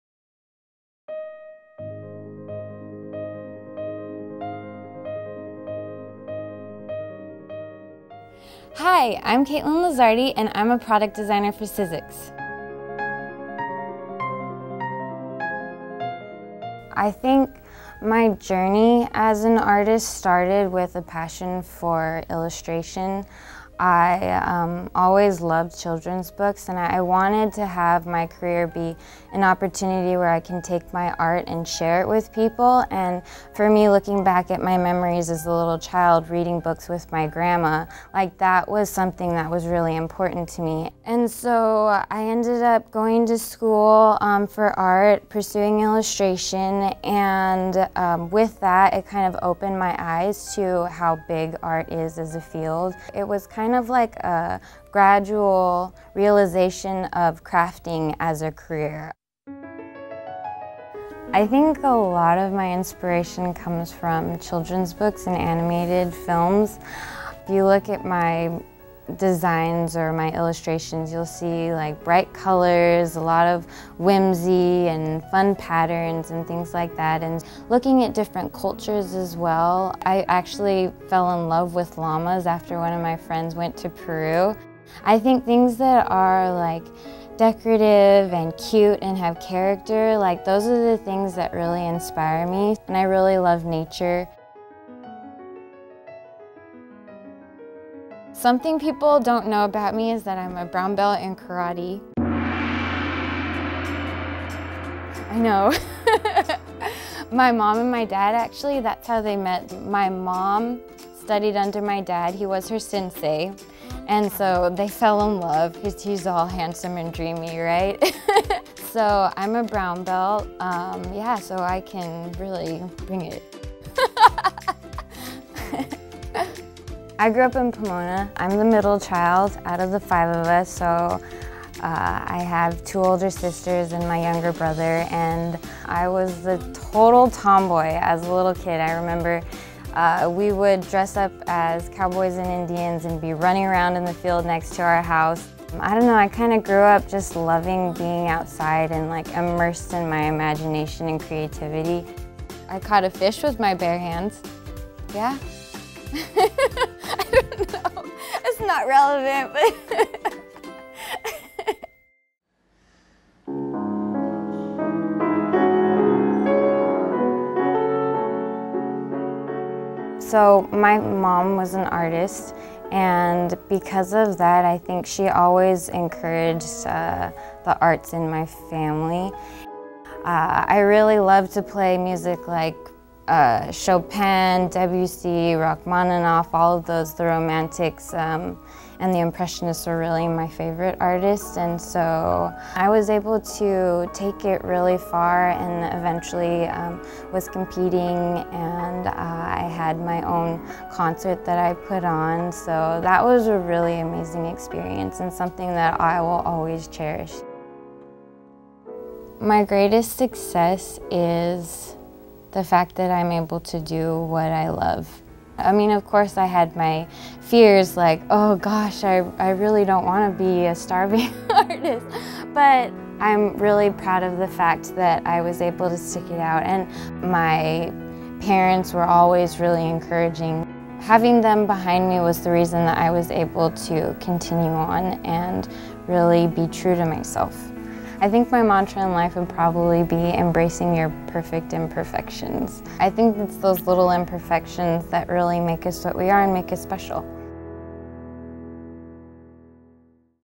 Meet the Designer: Katelyn Lizardi - Sizzix
Meet Sizzix designer, Katelyn Lizardi! Katelyn draws inspiration from her childhood, growing up reading children's books with her grandmother. She loves bright colors, whimsical drawings and of course llamas! Learn more about Katelyn and where she gets her inspiration from to create such beautiful designs that are shared with crafters like you!